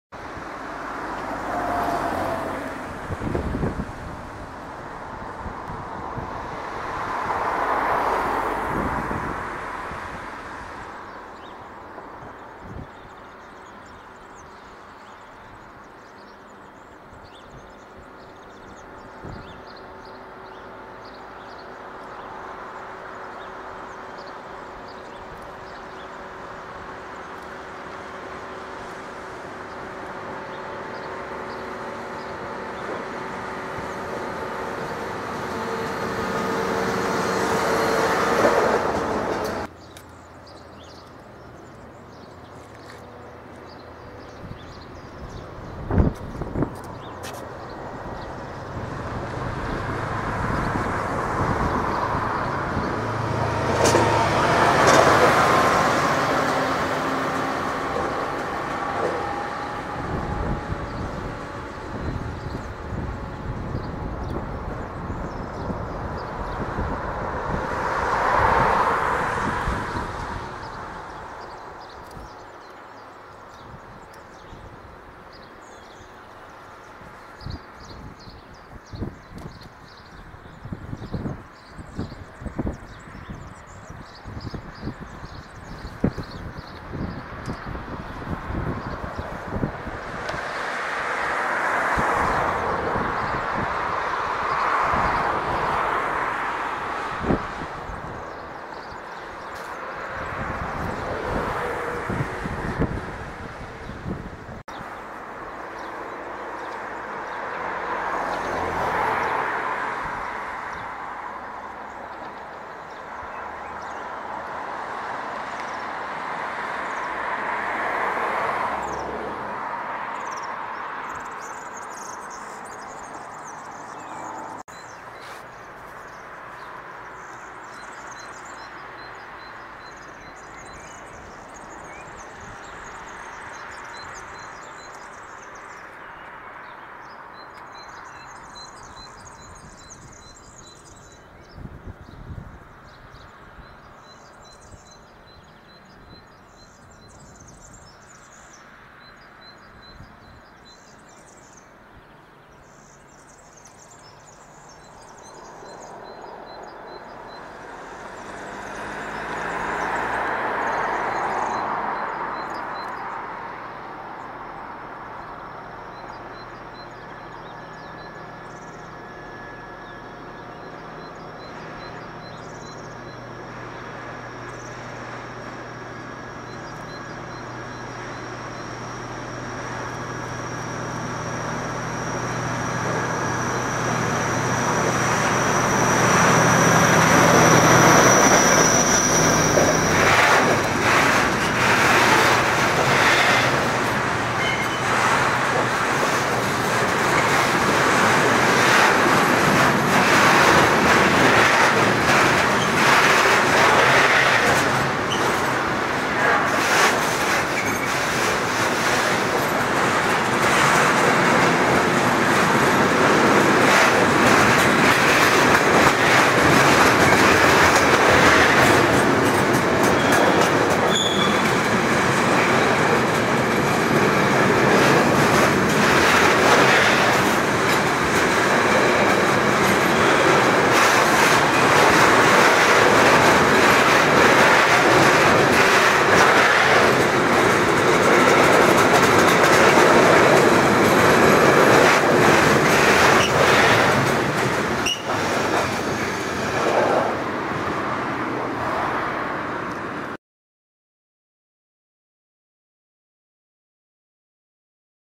Salzburger Eisenbahn Transportlogistik D VPS 92 80 1277 011 mit Hackschnitzelzug in Wiesau am 29 04
Salzburger Eisenbahn Transportlogistik D-VPS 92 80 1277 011 mit Hackschnitzelzug in Wiesau am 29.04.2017.